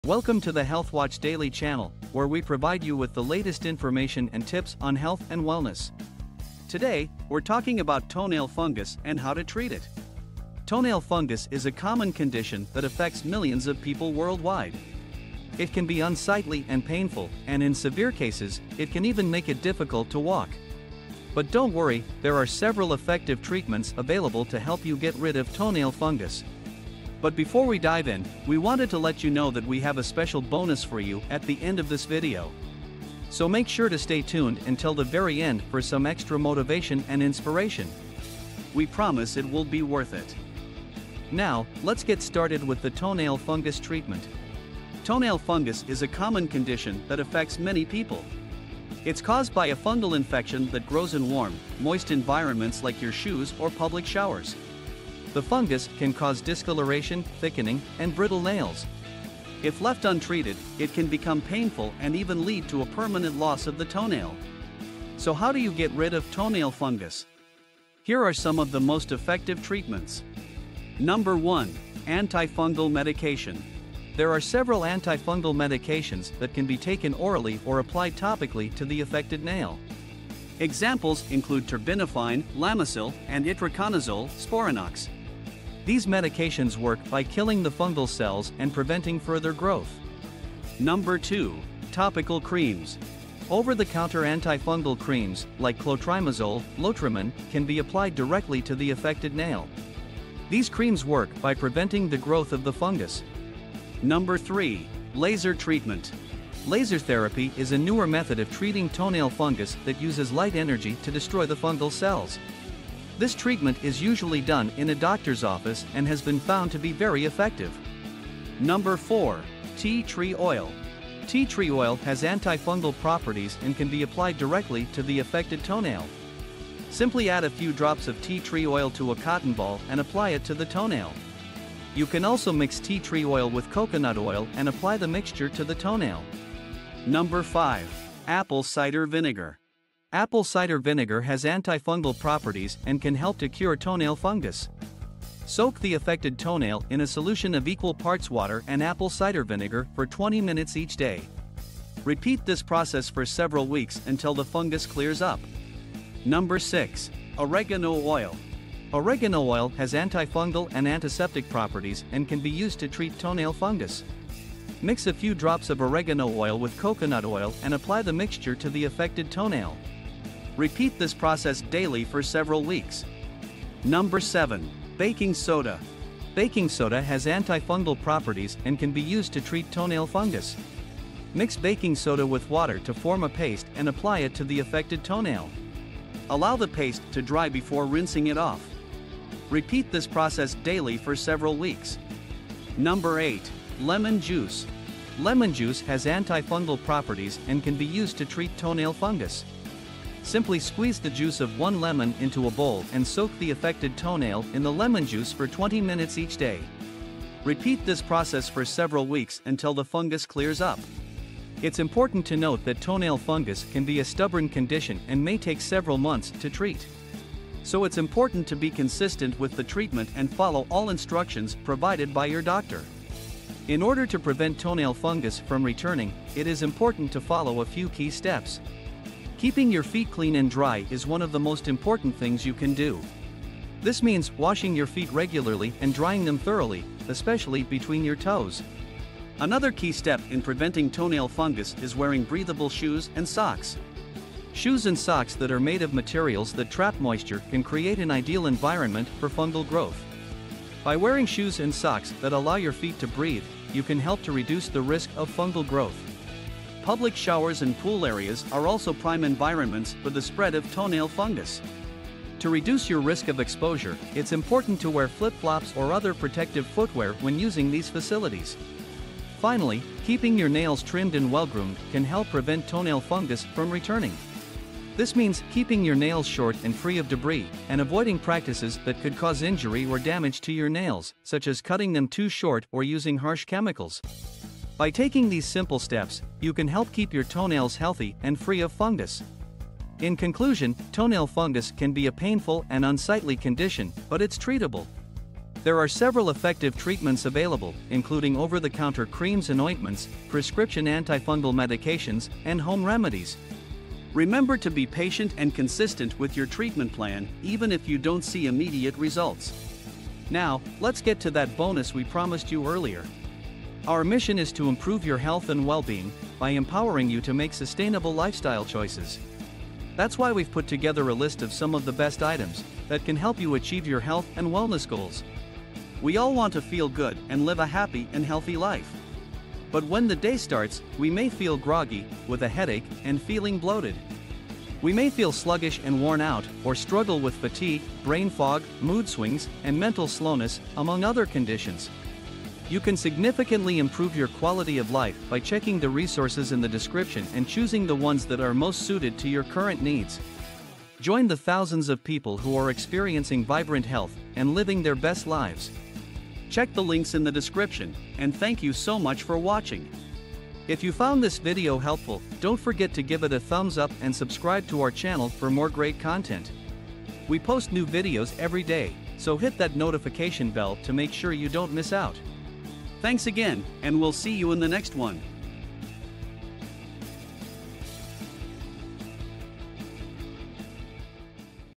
How to Beat Toenail Fungus once and for all - You won't BELIEVE the Solution!
Did you know that toenail fungus is one of the most common disorders? In this video, we're going to show you the easiest and most effective toenail fungus treatment that you'll ever try.

The fungal infection is caused by the fungus Candida Albicans, and it's a very common condition. In this video, we're going to discuss the different types of toenail fungus, how to identify them, and how to treat them with the help of natural home remedies. After you watch this video, you'll know how to get rid of toenail fungus for good!